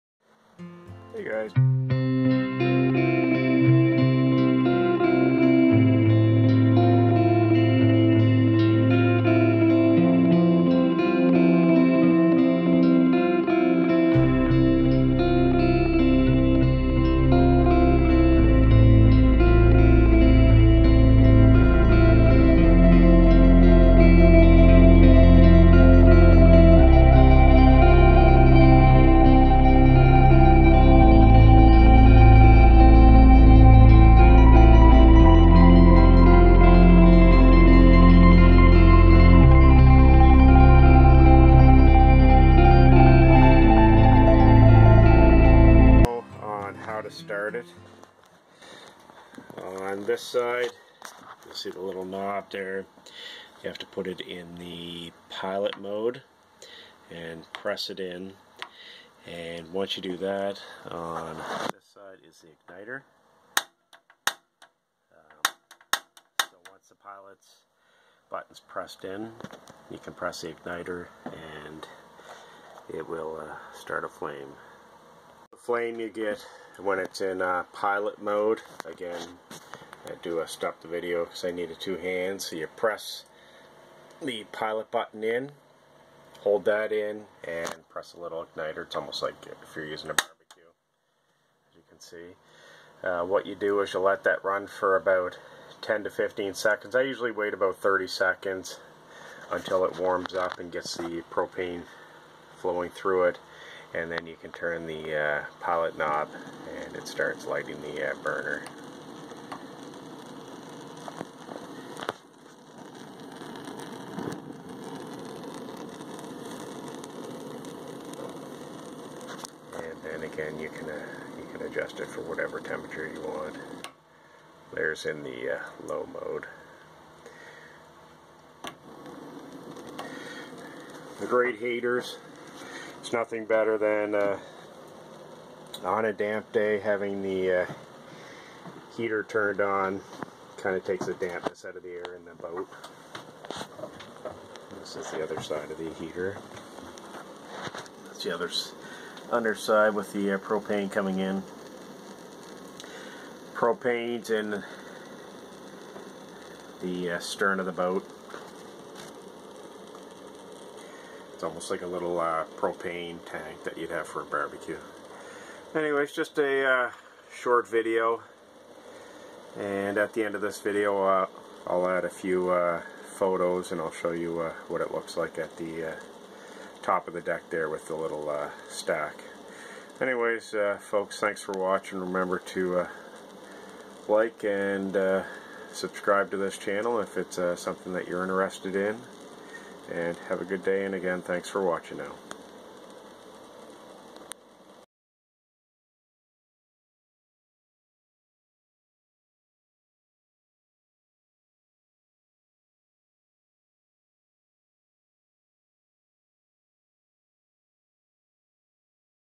Force 10 Propane Fueled Model 10000 Sailboat Yacht Heater
This is a short video of our Force 10 Propane fuel model 10000 cozy cabin heater that is in our 38-foot C&C sailboat.